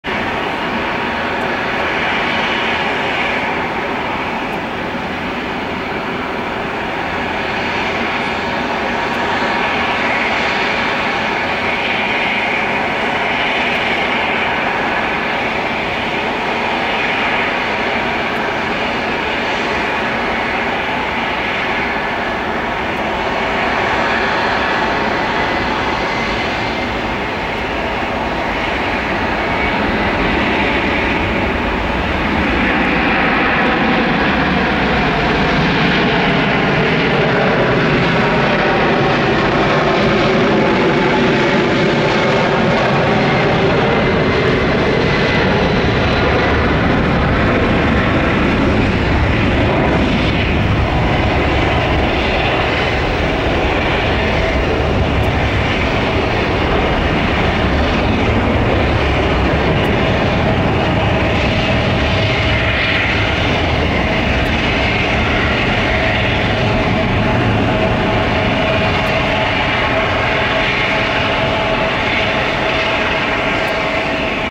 UPS N167UP A300 Takeoff Portland Airport (PDX)
Flight: UPS973 PDX-SDF